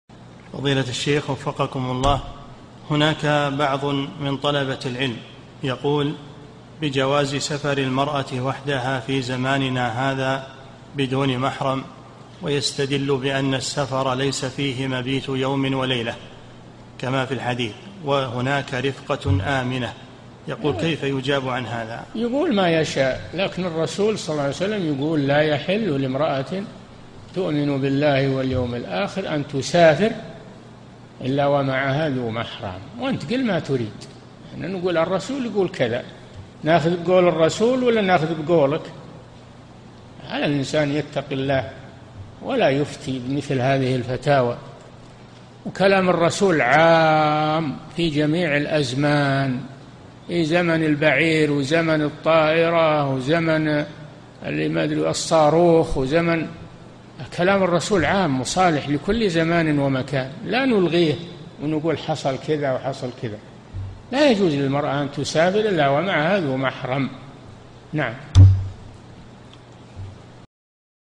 [769 -3022] بعض طلبة العلم يقول: يجوز للمرأة أن تسافر وحدها في زماننا هذا دون محرم، فما الجواب عليه؟
-"[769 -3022] السؤال: هناك بعض من طلبة العلم، يقول بجواز سفر المرأة وحدها في زماننا هذا بدون محرم، ويستدل بأن السفر ليس فيه مبيت يومٍ وليلة، كما في الحديث وهناك رفقةٌ آمنة، يقول كيف يجاب عن هذا؟
الجَوَابُ: يقول ما يشاء، لكن الرسول ﷺ يقول: لا يحل لامرأةٍ تؤمن بالله واليوم الآخر أن تسافر إلا ومعها ذو محرم، وأنت قل ما تريد، نحن نقول الرسول يقول كذا، نأخذ قول الرسول ولا نأخذ بقولك؟ على الإنسان يتقي الله، ولا يفتي بمثل هذه الفتاوى، وكلام الرسول عام في جميع الأزمان، في زمن البعير، وزمن الطائرة، وزمن الصاروخ وزمن. كلام الرسول عام صالح لكل زمان ومكان، لا نلغيه ونقول حصل كذا، وحصل كذا، لا يجوز للمرأة أن تسافر إلا ومعها محرم، نعم.
للاشتراك في قناة الشيخ صالح الفوزان

للاستفسار والإقتراحات والدعم
رئيس الفريق
لمشروع كبار العلماء